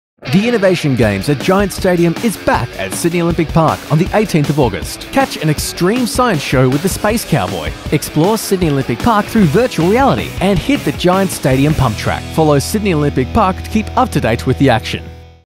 The Innovation Games return to Sydney Olympic Park
Sydney Olympic Park will be buzzing with drone simulations, big science shows, wildlife demonstrations and more! at Sydney Olympic Park INNOVATION AGMES 2019!
We'll be bringing science to sports fans attending the GWS GIANTS AFL game at GIANTS Stadium.